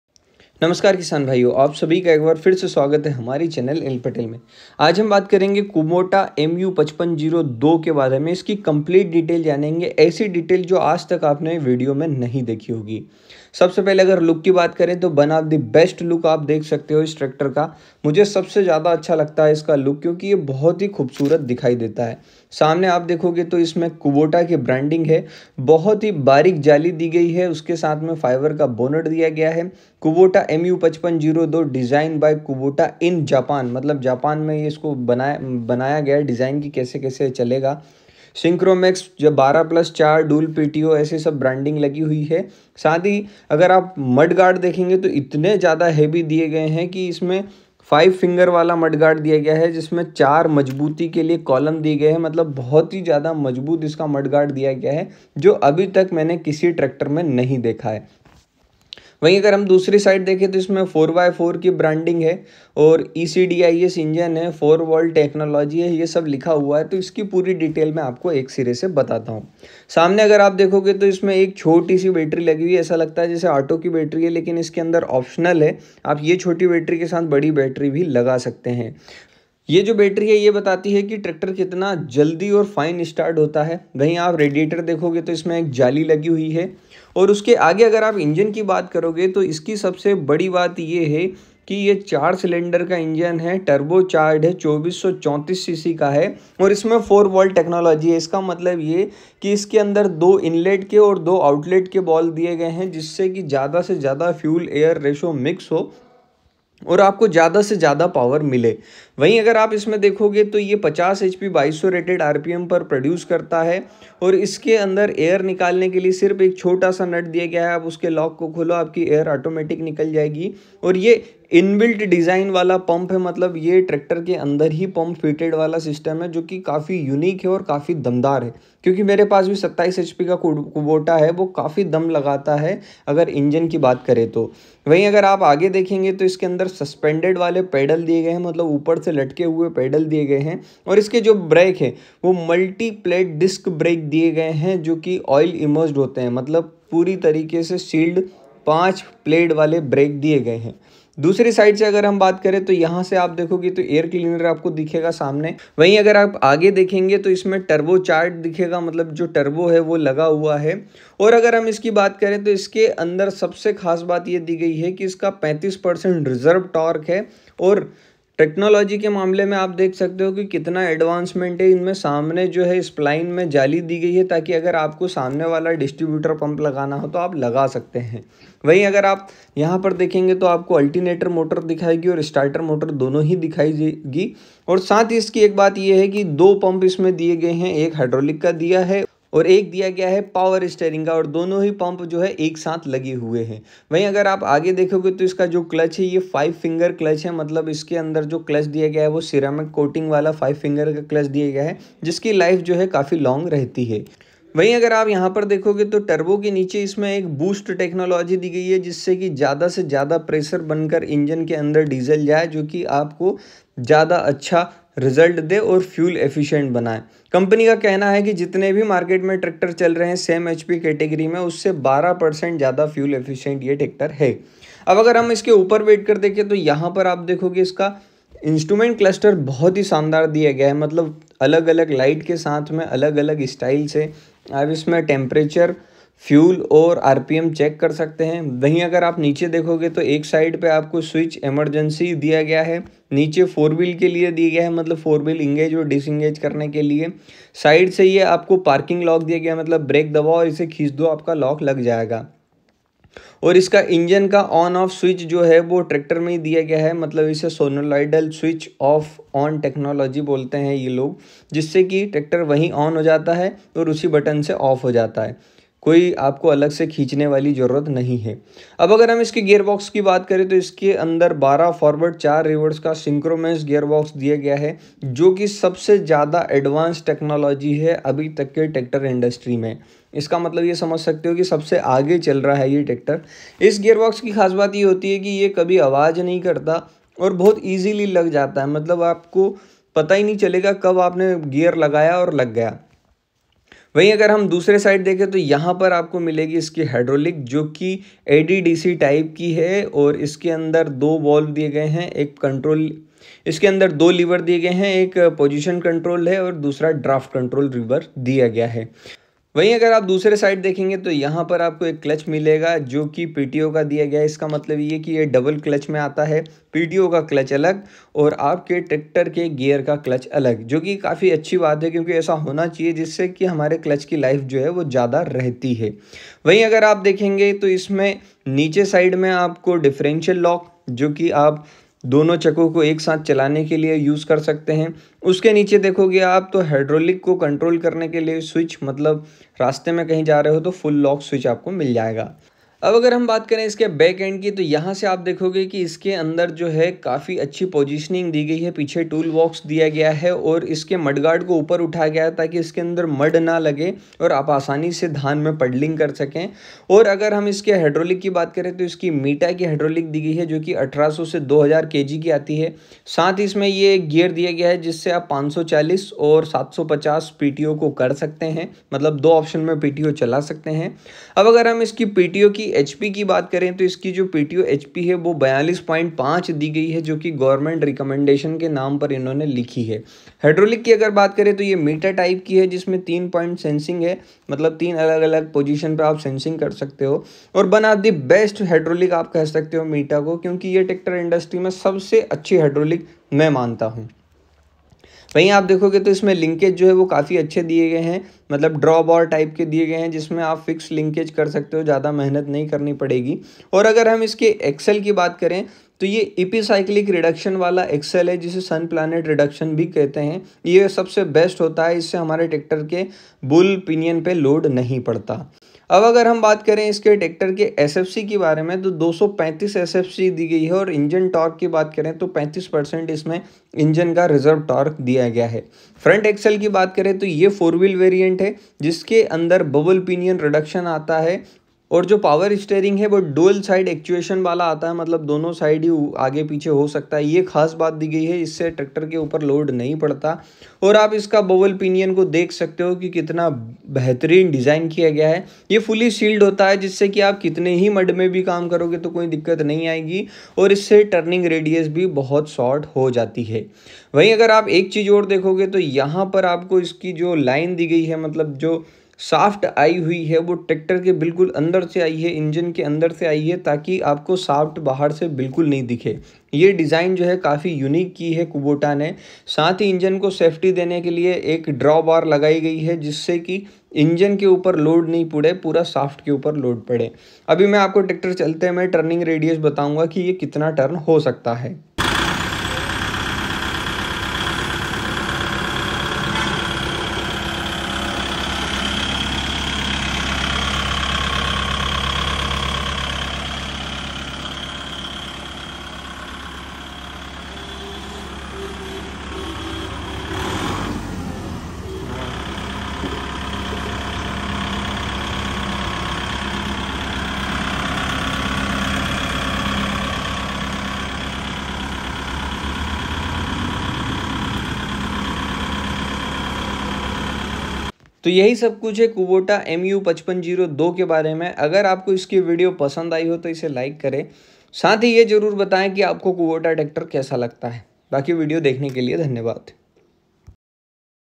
New 2024 Kubota Mu 5502 Tractor full review and specifications|पुरे भारत मे 55 hp बेस्ट ट्रैक्टर
Technical Specifications

Kubota MU5502 4WD tractor specification helps you to decide what to buy and easier for you to compare with another tractor brands.

￼BrandKubota￼ ModelMU5502 4WD￼ Name Of EngineE-CDIS￼ HP50￼ Displacement-CC2434 CC￼ Engine-RPM2100 RPM￼ Specific Fuel Consumption235 g/kW/h￼ Transmission TypeSynchromesh Tranmission￼ Number Of Gears12 Forward + 4 Reverse￼ Clutch TypeDual Clutch￼ PTO TypeDual￼ PTO Speed540/750